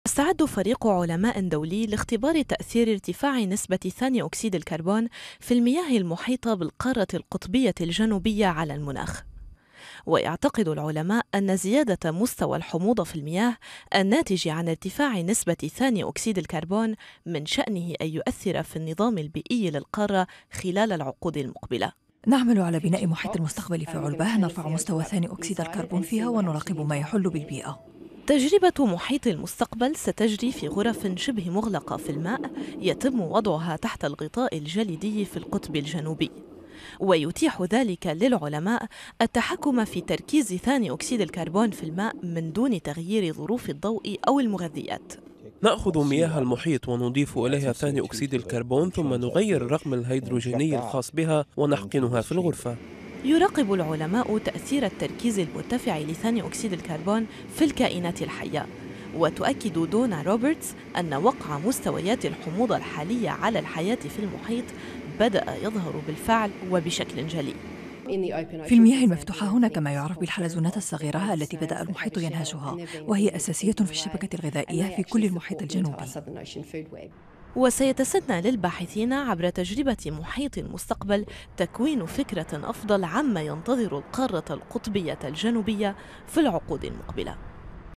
اختبار لمناخ أنتاركتيكا
يعمل خبراء دوليون على اختبار وقع ارتفاع تركيز ثاني أوكسيد الكربون في مياه المحيط على مناخ القارة القطبية الجنوبية. ويأمل هؤلاء في تكوين فكرة واضحة عن مدى تأثير ذلك في النظام البيئي للقارة وحياة مختلف الكائنات.